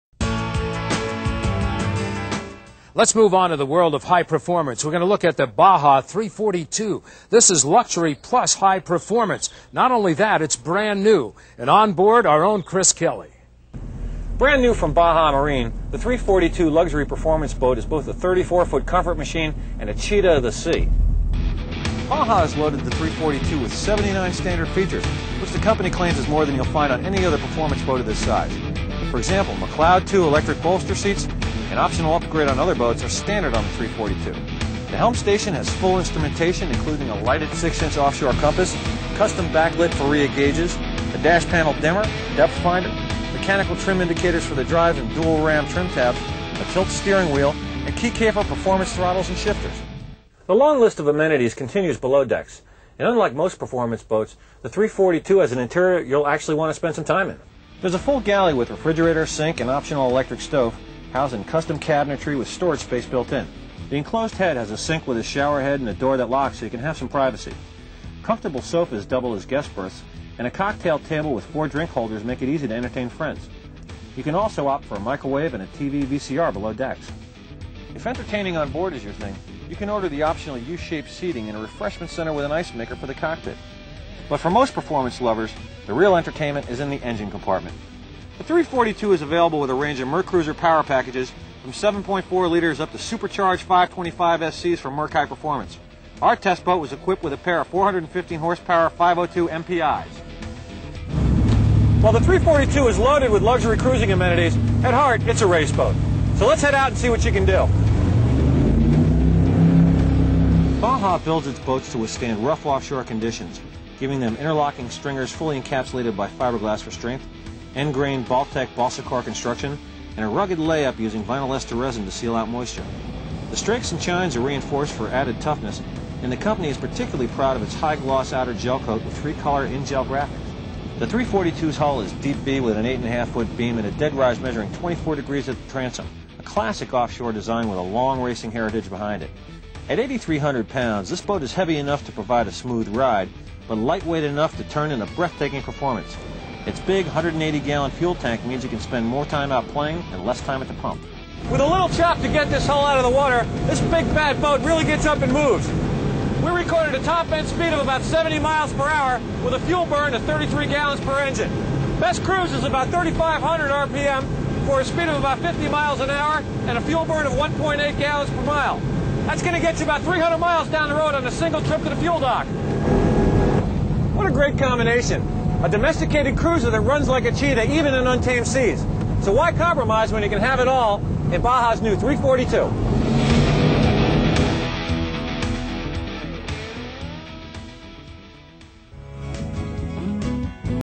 Baja 342 as tested by BoatTEST.com
The 342 is the entry-level boat in Baja's "luxury performance" series, and it's a good place to start for anyone new to- or moving-up in performance boating.